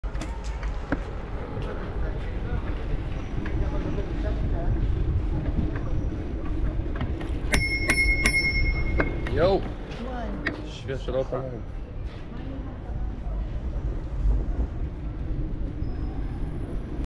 Old Man Walks in the Bike Lane
This was a very close call in the Tel Aviv bike lanes. This happens way too often in this city - people just do not look before they walk in the bike lanes.